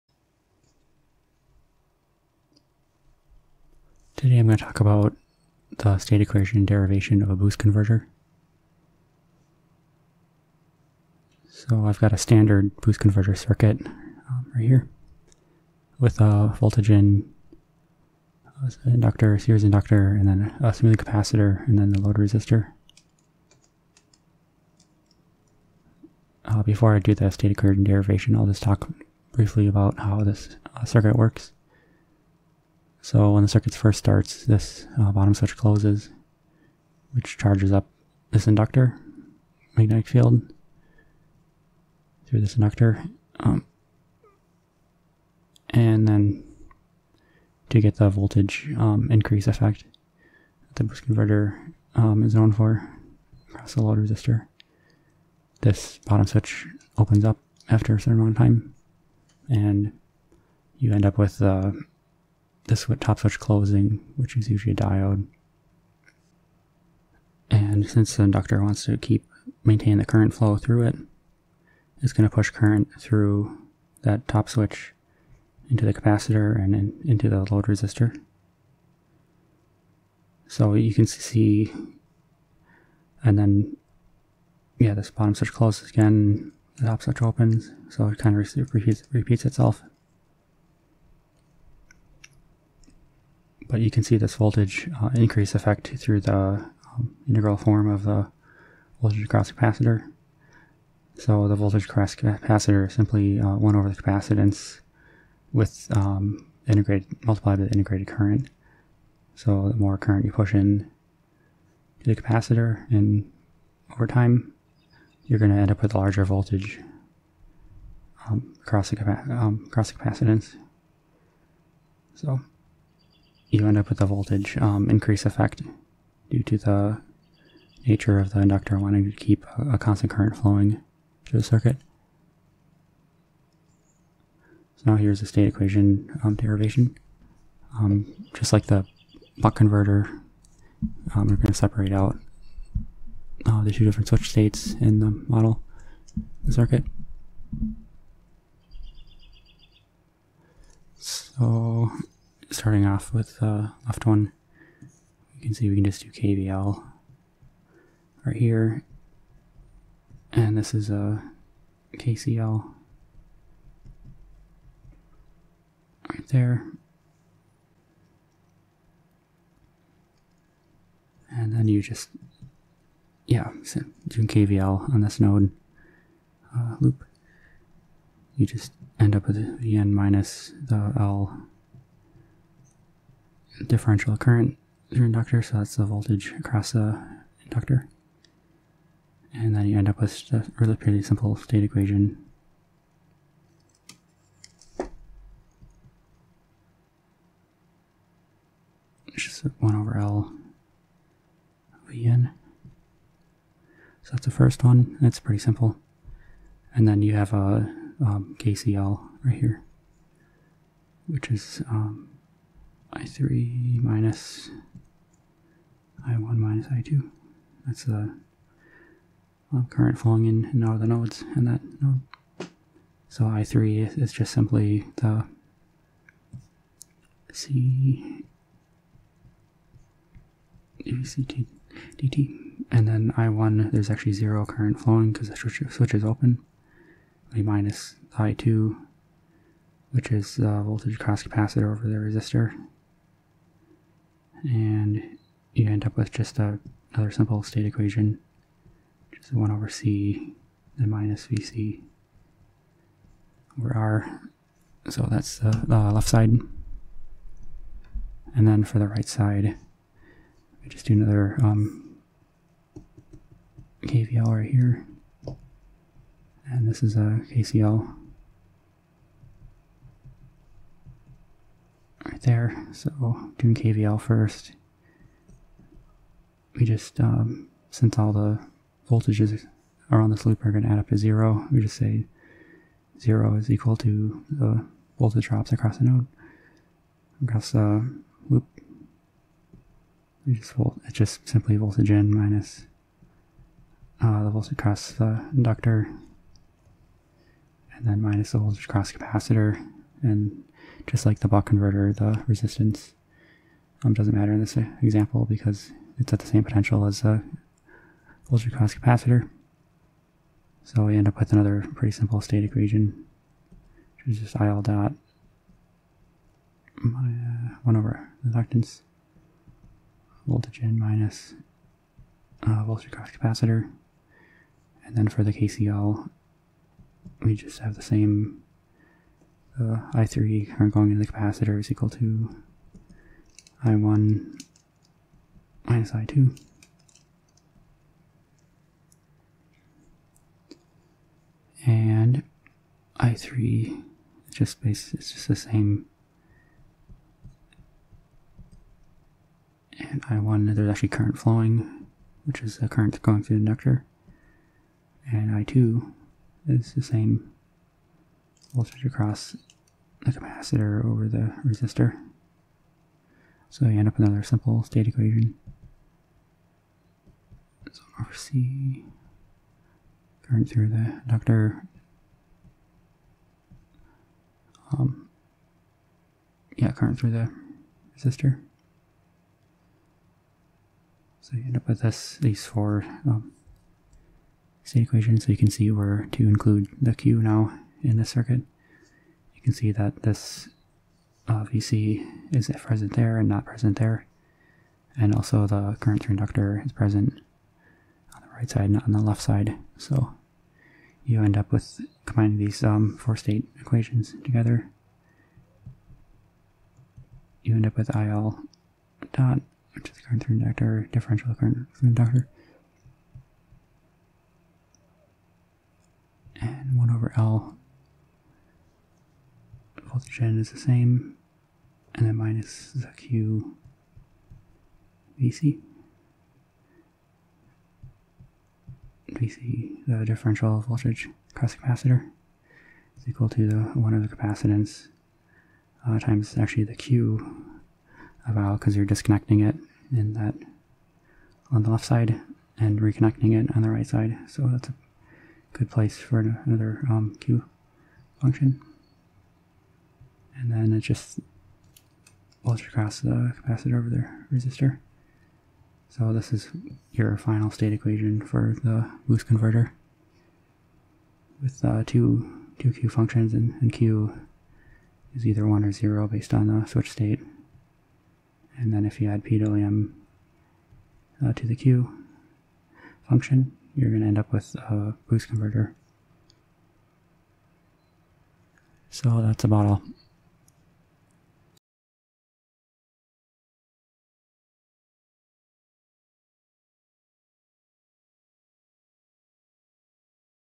Boost Converter State Equation Derivation
In this video, I describe how a boost converter works. I derive the state equation using KVL and KCL.